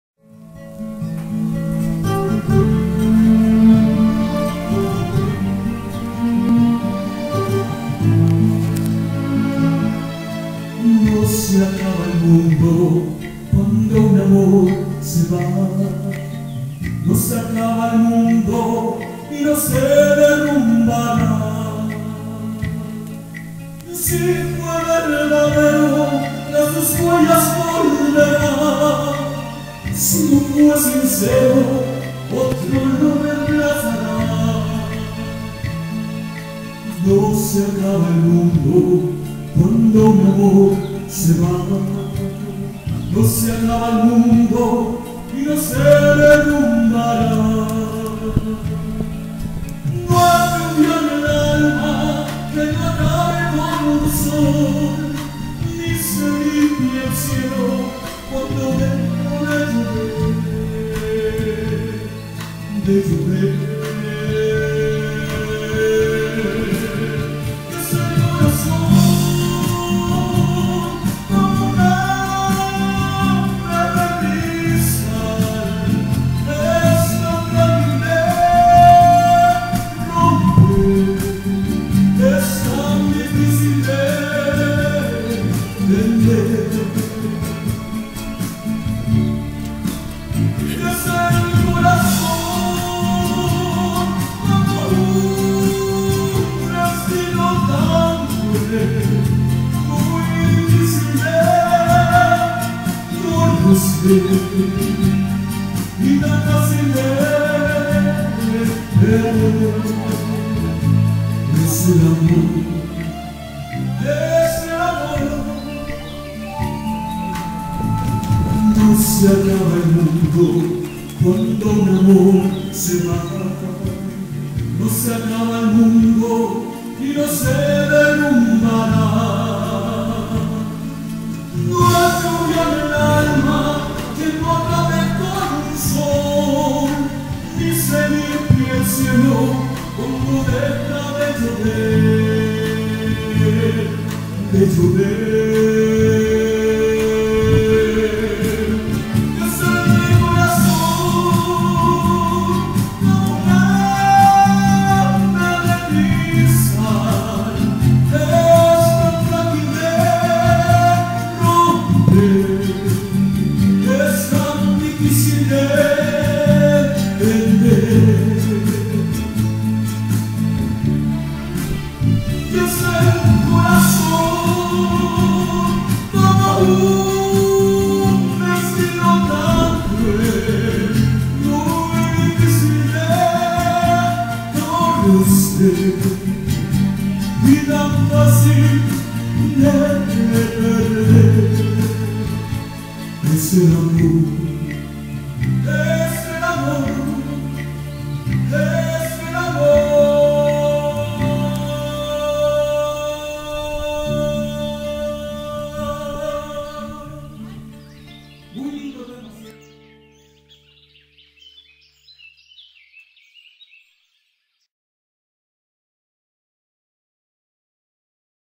AVE DE CRISTAL CANTA DAVOR EN SOCOROMA
27 de octubre 2010
Municipalidad de Putre
Día internacional del Adulto Mayor